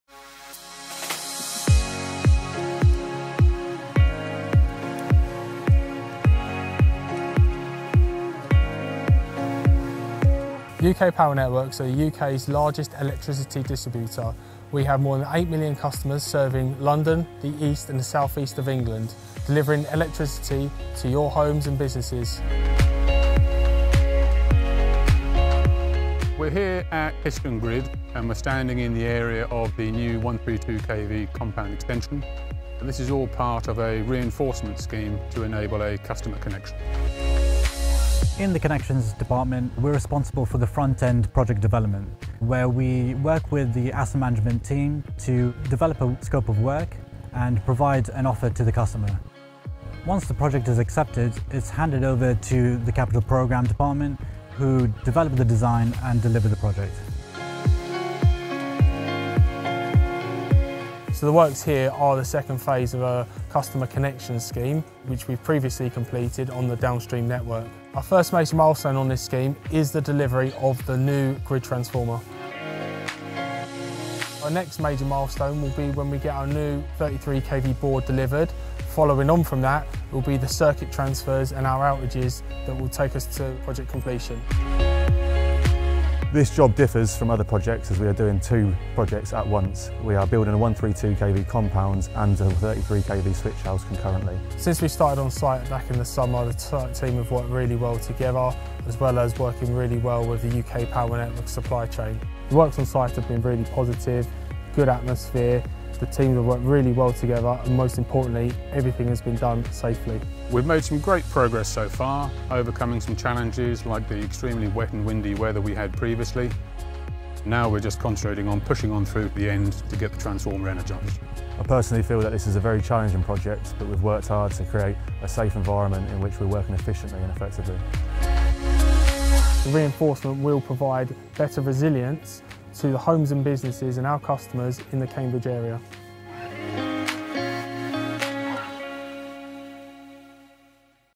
Histon Grid Reinforcement Project
This project is focusing on installing a new grid transformer and a new 33kv switchboard. This is to reinforce our existing electricity network.